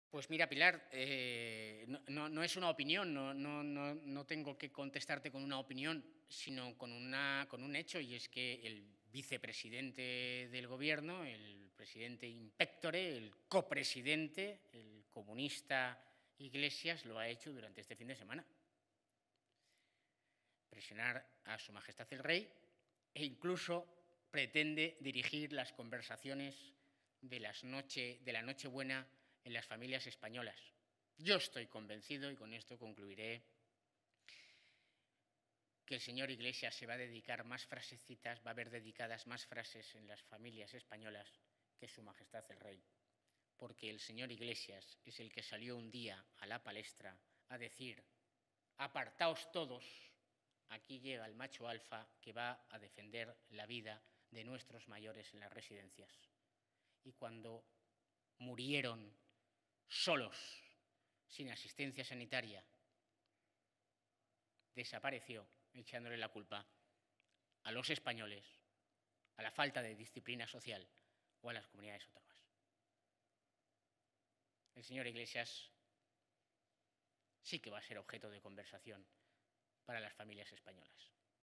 Vox cree que el Rey hará un discurso "con sentido común y nacional"
El portavoz del Comité de Acción Política de Vox, Jorge Buxadé, augura que el Rey hará en Nochebuena un discurso "con sentido común y nacional" pese a las "presiones" que, a su juicio, está ejerciendo el vicepresidente segundo del Gobierno y líder de Podemos, Pablo Iglesias, al que ha acusado de pretender "dirigir" las conversaciones de los españoles estas fiestas navideñas.